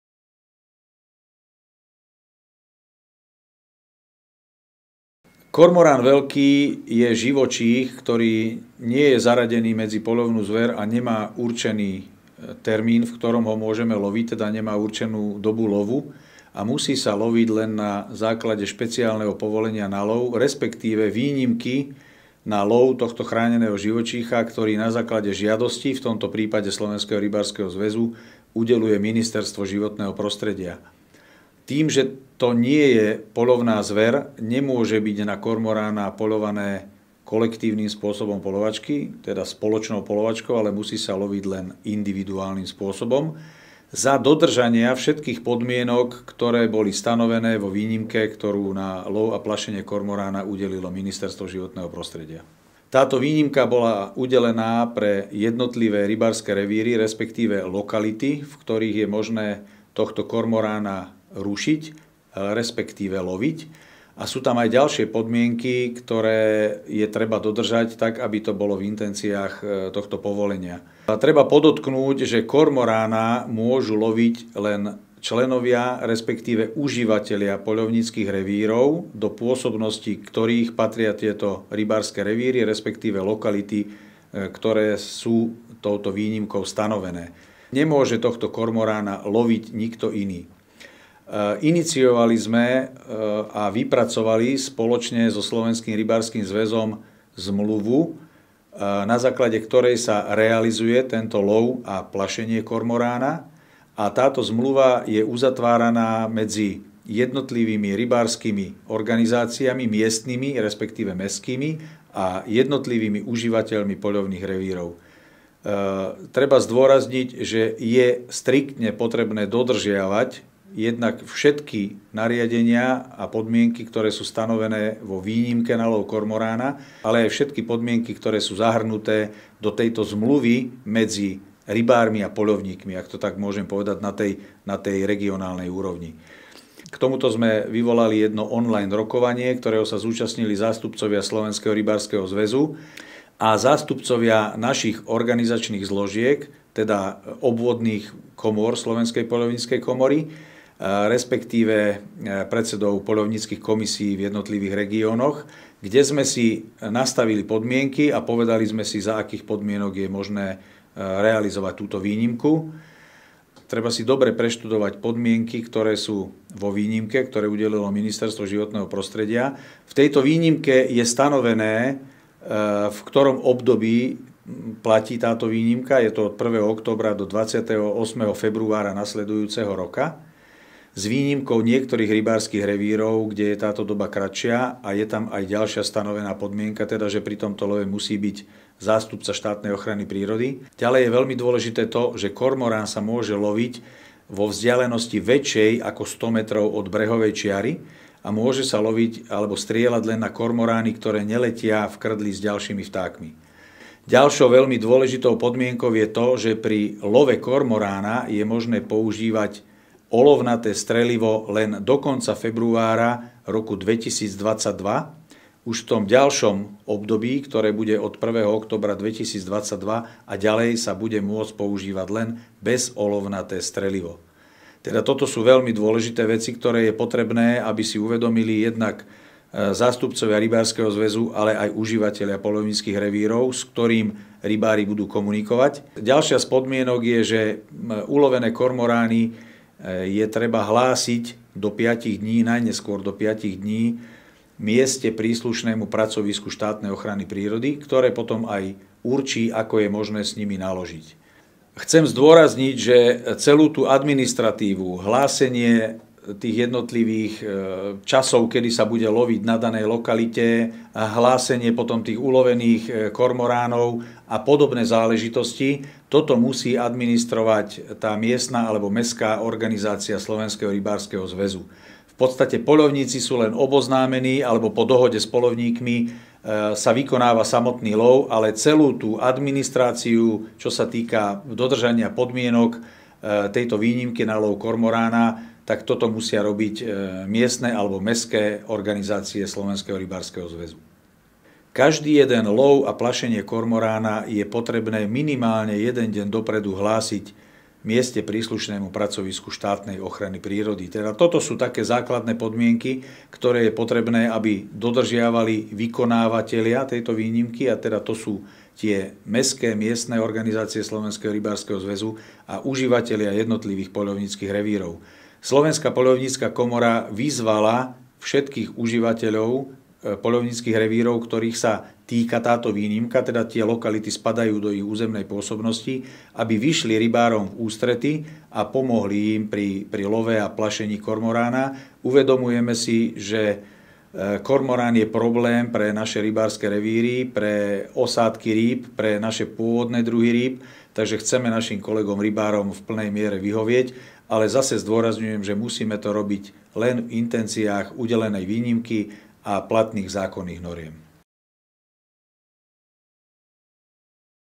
RTV EXTRA : Kto, kedy a ako smie loviť kormorána veľkého
Po takmer dvojročnej prestávke sa na Slovensku opäť môže plašiť a loviť kormorán veľký. Prečo a za akých podmienok je možné strieľať tohto predátora si môžete pozrieť v špeciálnom vysielaní RTV Extra s pozvaným hosťom, Ing. Alojzom Kaššákom, viceprezidentom Slovenskej poľovníckej komory.

NEZABUDNITE dať lajk a zdieľanie, ak sa vám relácia páčila a zanechať komentár. Nech sa vám darí. Petrov Zdar!